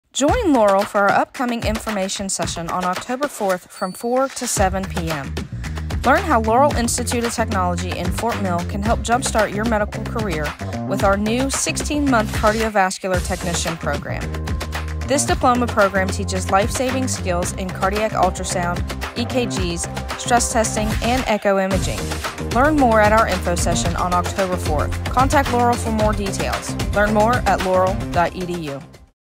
LIOT Information Session 10.4
Join Laurel for an information session on October 4th from 4 - 7 PM and learn about our Cardiovascular Technology program!